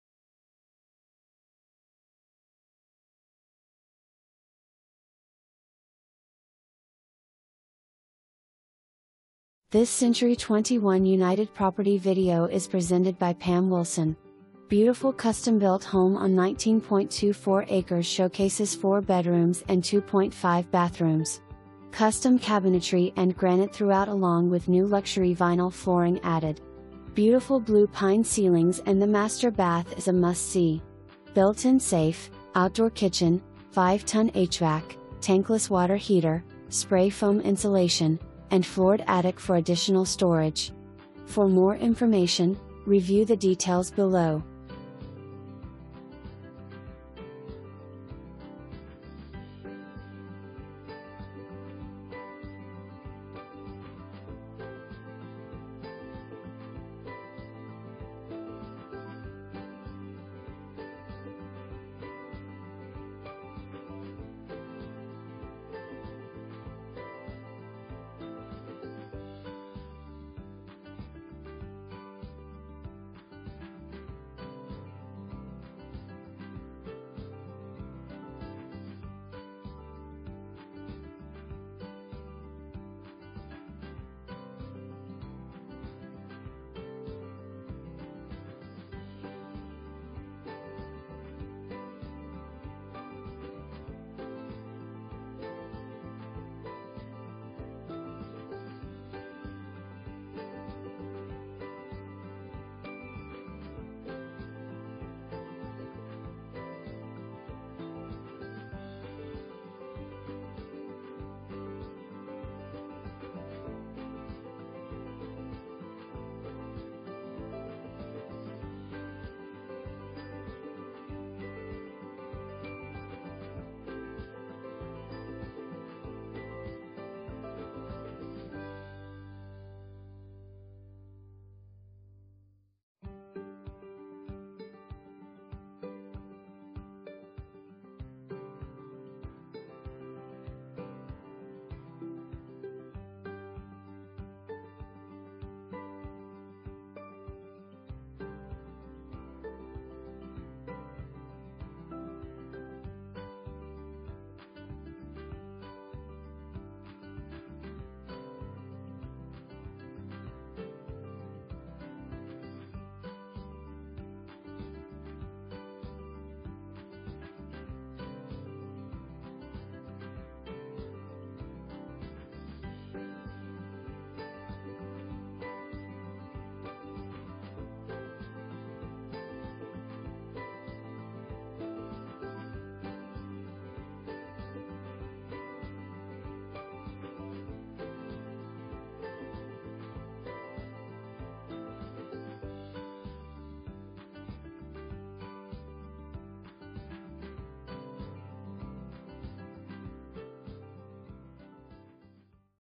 CENTURY 21 United - 612 Willett Rd.
612 Willett Rd.

Beautiful custom-built home on 19.24 acres showcases 4 bedrooms and 2.5 bathrooms. Custom cabinetry and granite throughout along with new Luxury Vinyl flooring added. Beautiful blue pine ceilings and the Master Bath is a must see!! Built-in safe, outdoor kitchen, 5 Ton HVAC, tankless water heater, spray foam insulation, and floored attic for additional storage.

3 full bath, 1 half bath

Pam Wilson
CENTURY 21 United